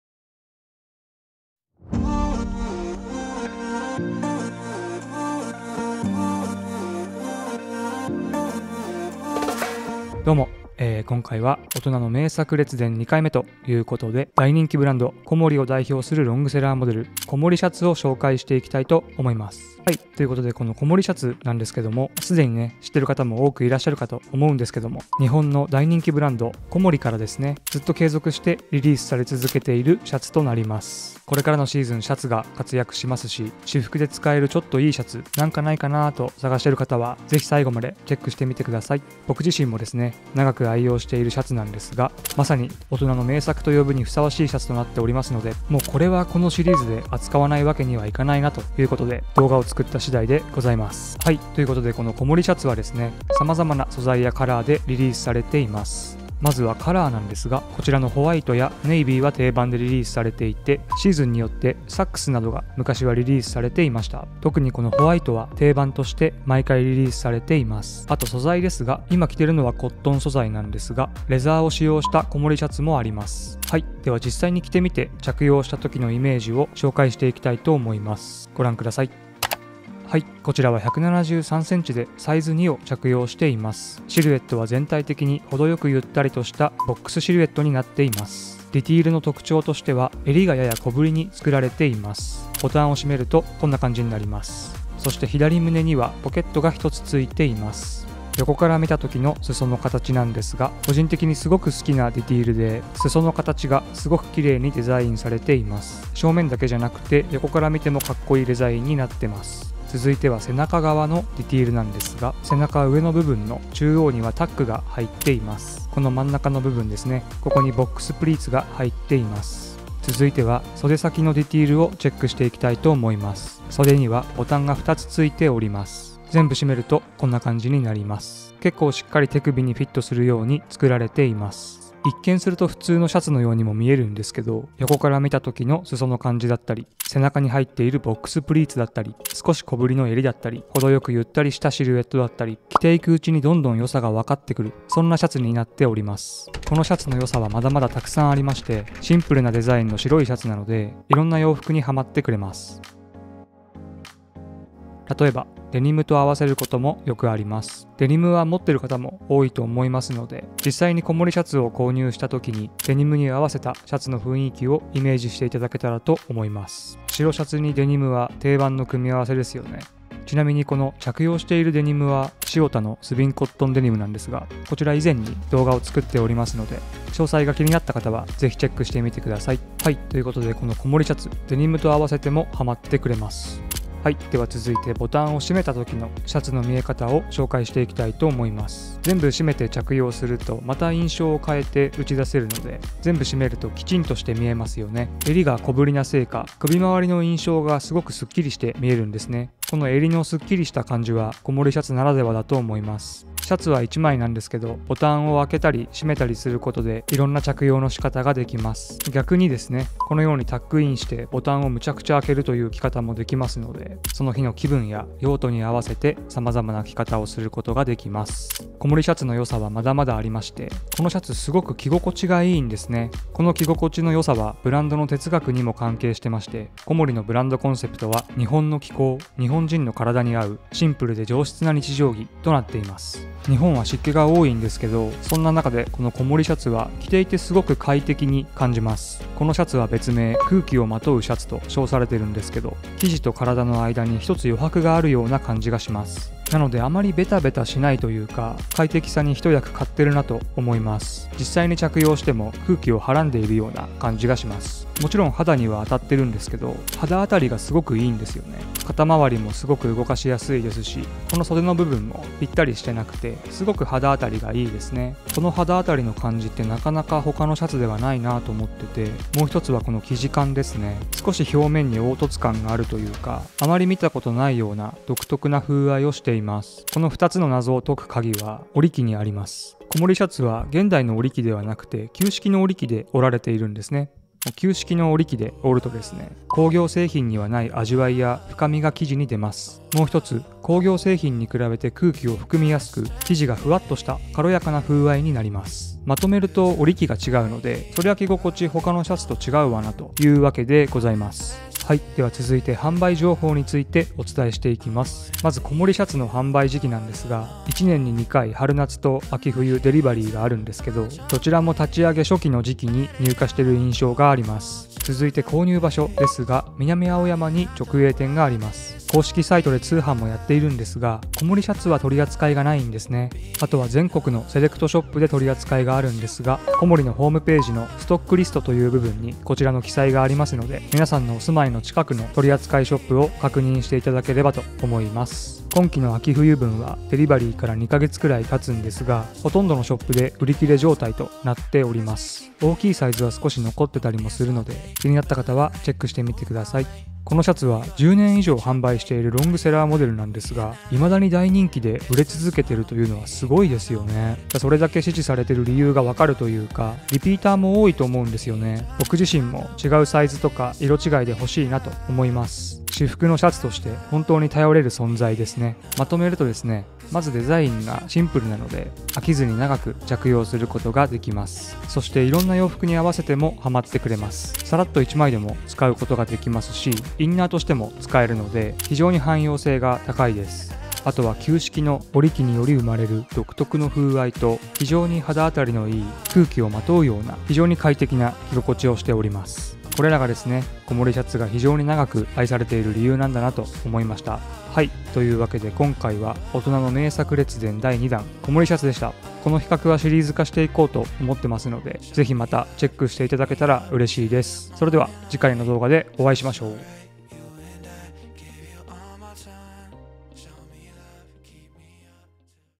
COMOLI コモリシャツ 【大人気ブランドのロングセラー】
0:00~1:19 コモリシャツについて
1:19~1:28 シルエットについて
1:28~2:11 ディテールについて
2:11~2:33 コモリシャツの良さ
2:33~3:03 色々な服にハマるコモリシャツ
3:03~3:38 様々な着方
3:38~5:01 着心地・生地について
5:01~5:36 販売情報について
5:36~6:04 コモリシャツの人気ぶりについて
6:05~6:52 まとめ